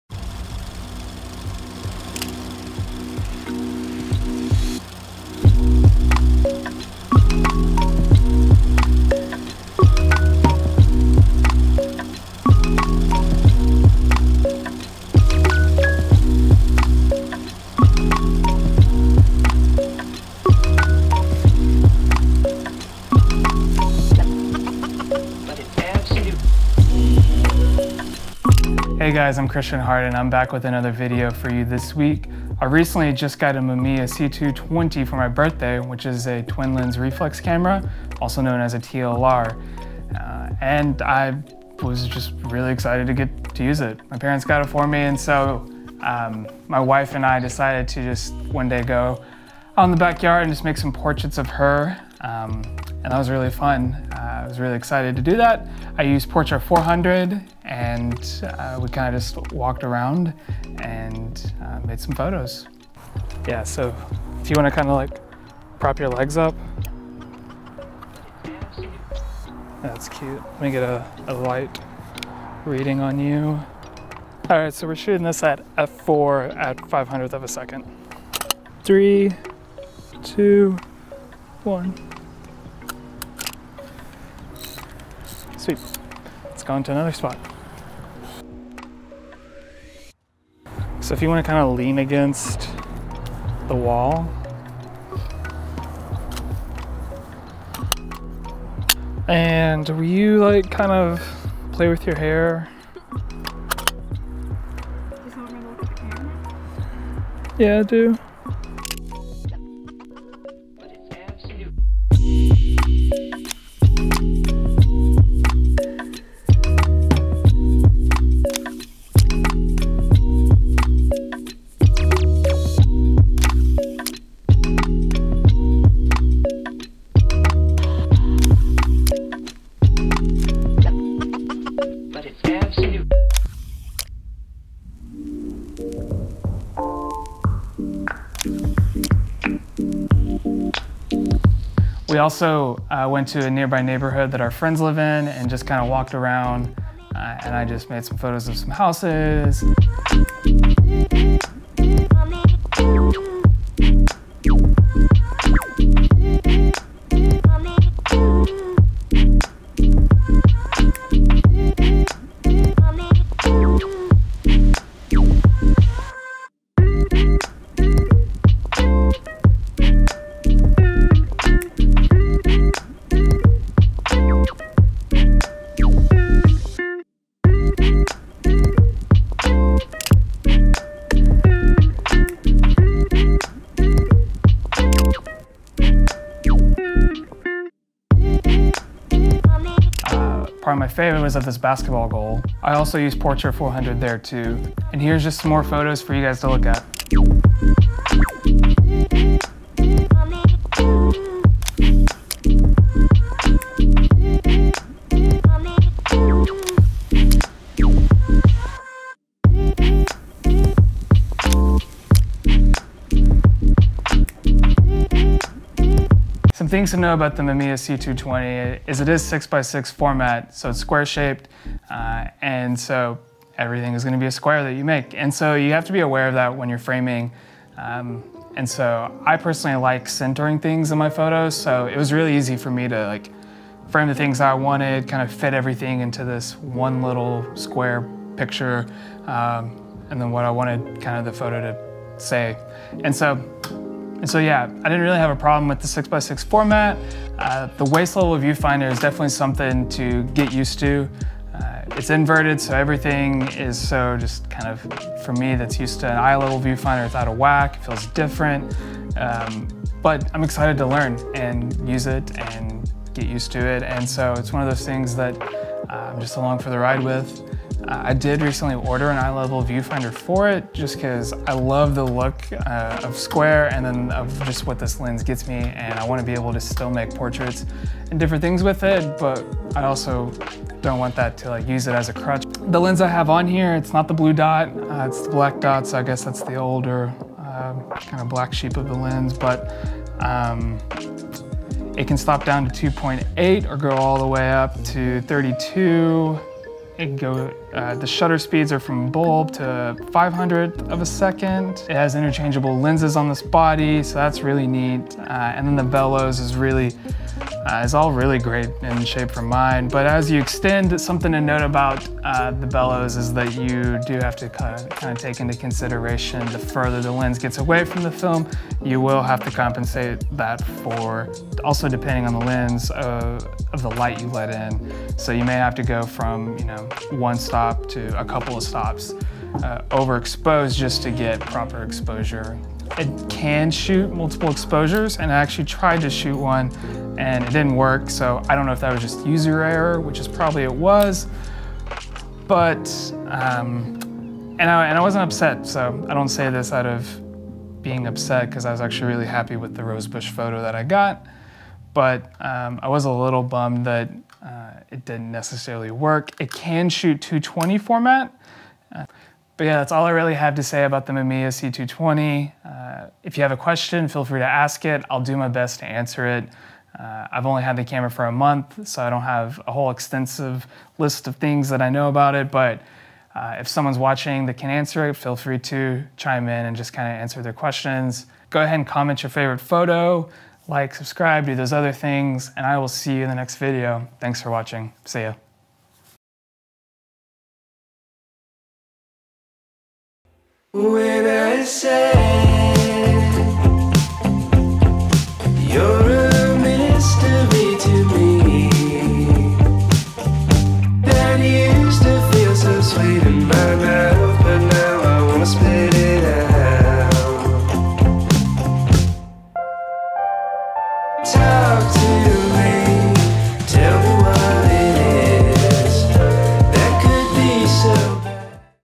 Shooting Portraits on the Mamiya C220
From photos at my house, to a walk in the neighborhood, this video has a solid variety of photos.

Comment your favorite!

MB01ZYSY9DCPXHA